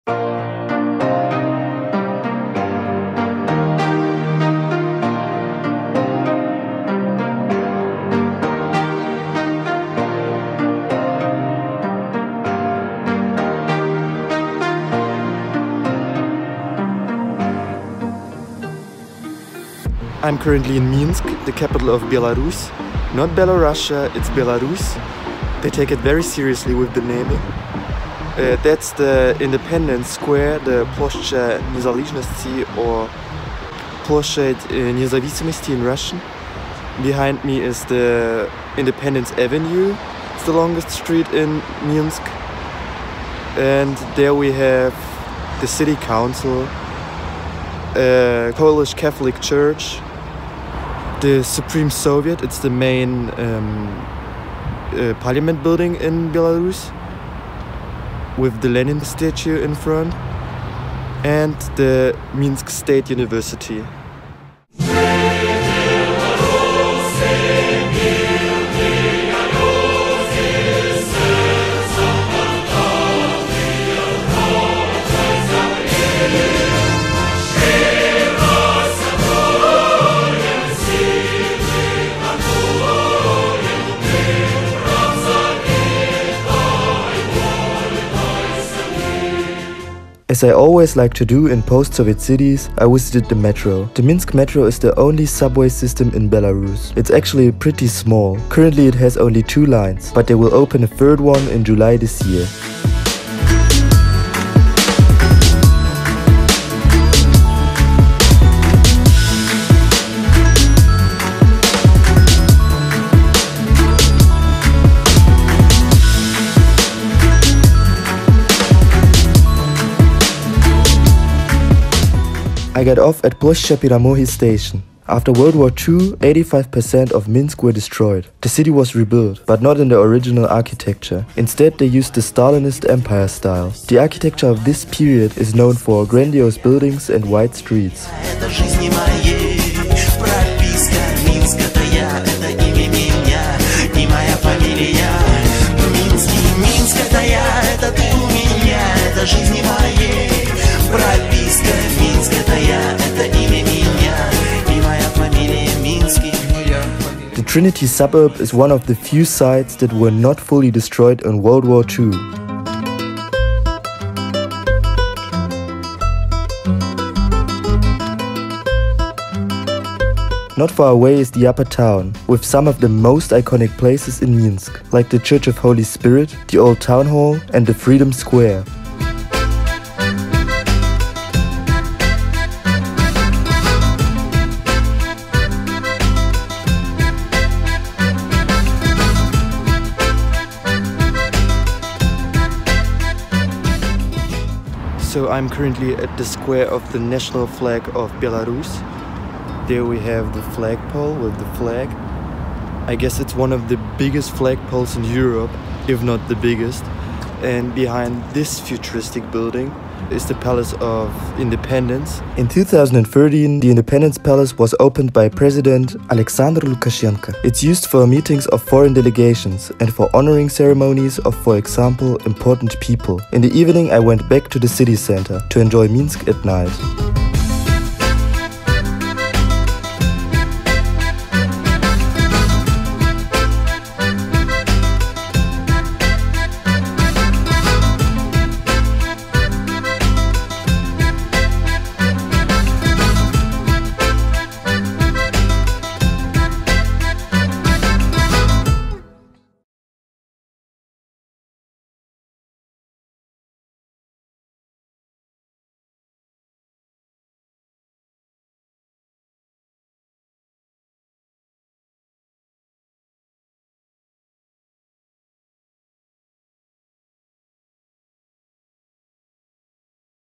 VLOG #028 \\ A tour of MINSK - EUROPE'S most UNDERRATED country - BELARUS! Is it worth visiting?
SEASON V | Minsk, Belarus
In January I visited the capital of Belarus - Minsk. The city is one of the cleanest I have ever seen in my entire life. People who are interested in socialist architecture will definitely like it. Minsk has many of those wide avenues and pompous stalinist buildings.